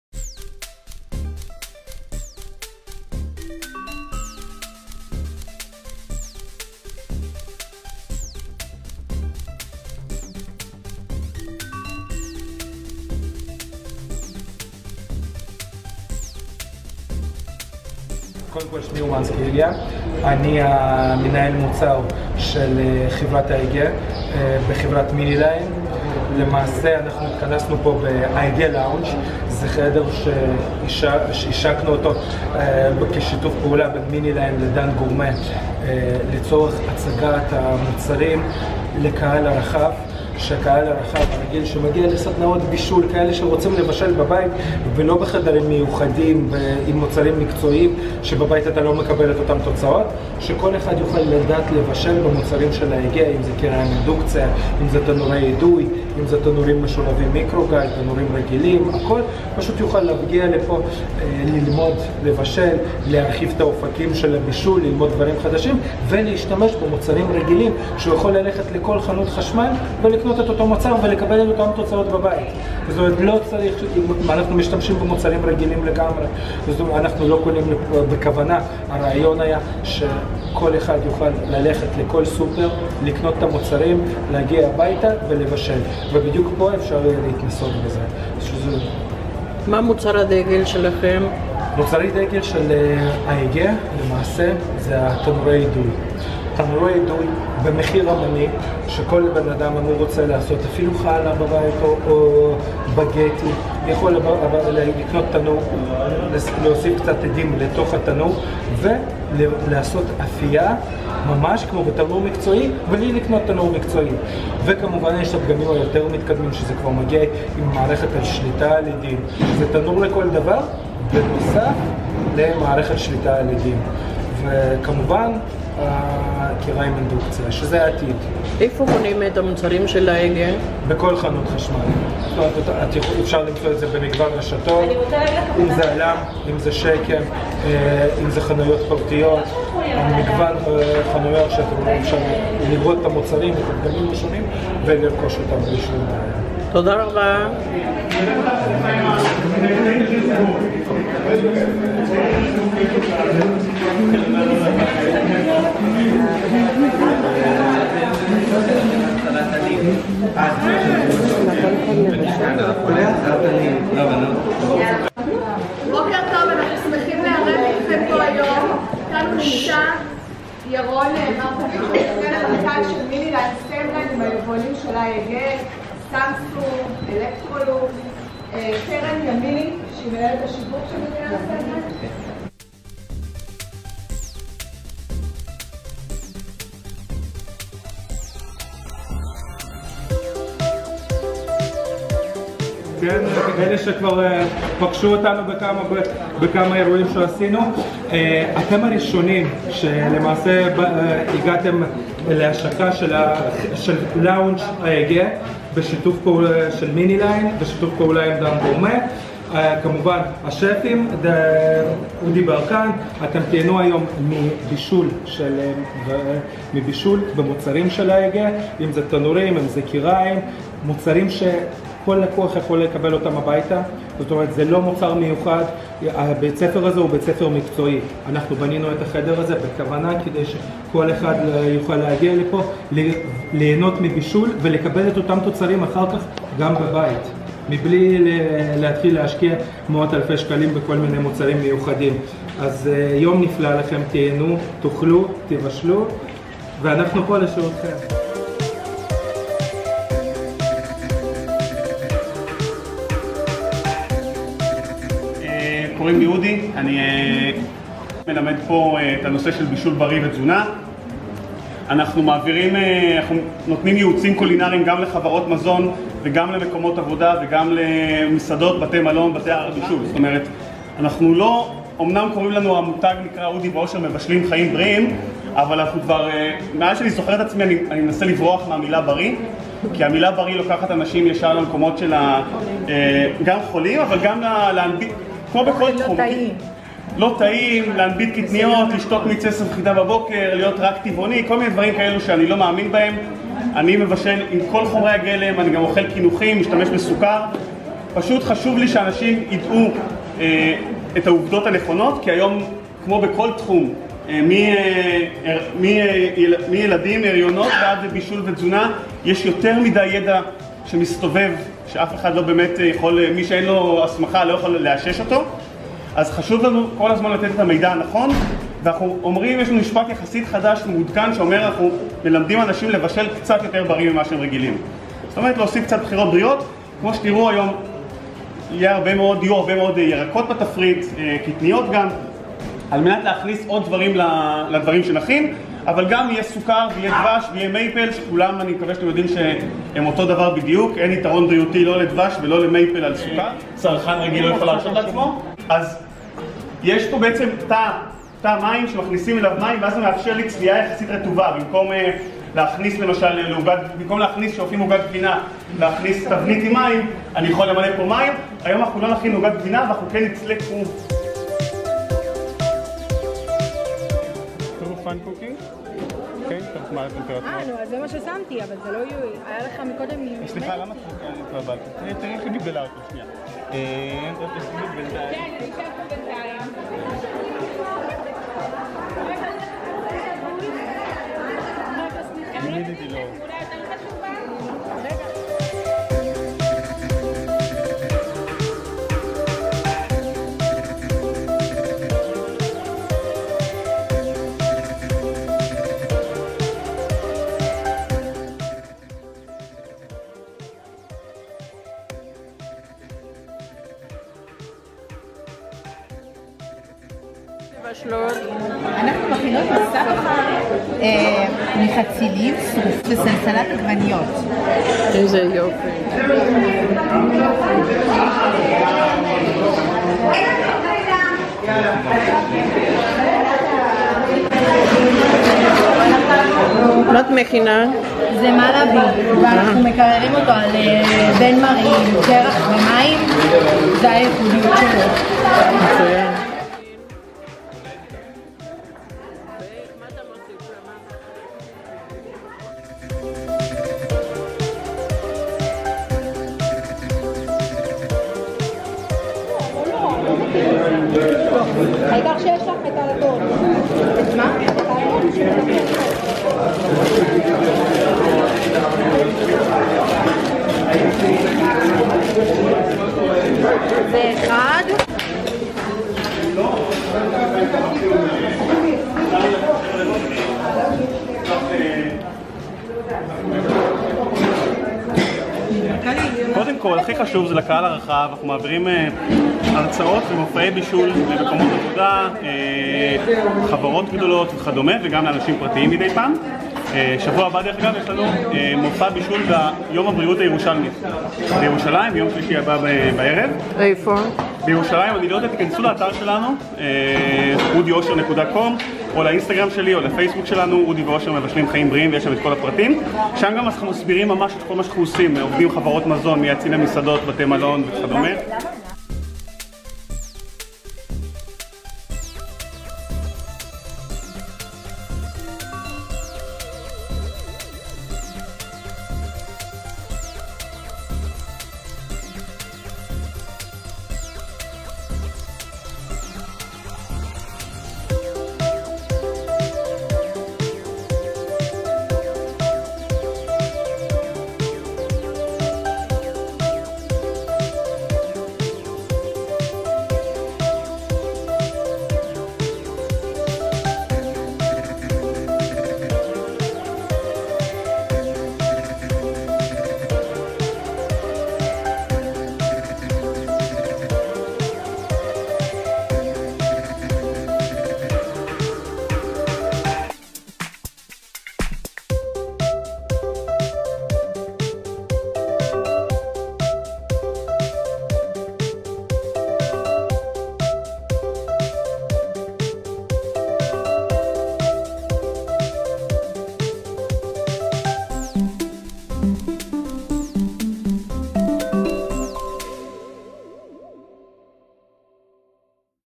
AEG MINILINE en colaboración con Dan Gourmet 2019
MINILINE lanza en colaboración con Dan Gourmet, AEG LOUNGE escuela de culinaria, para entusiastas de la gastronomía.

 El salón AEG contará con talleres de cocina donde los participantes podrán cocinar y experimentar con los productos de la prestigiosa  marca.
 Los productos del salón son para uso hogareño para que cualquier entusiasta de cocina pueda aplicar las lecciones que aprendió en la escuela culinaria Dan Gourmet en la propia cocina de los entusiastas culinarios.

La cooperación entre "MINILINE" y Dan Gourmet "se deriva de la comprensión de la creciente necesidad de talleres de cocina experimental en Israel para el cocinero casero.  Esta cooperación diseñada proporcionará soluciones tanto en los talleres de cocina y a los participantes. La experiencia de la exclusiva marca y los resultados sin compromiso con los productos de AEG, mientras que experimentan con una amplia variedad de productos de la marca  desde estufas, y pequeños productos tales como licuadoras, procesadores de alimentos, agitadores, etc.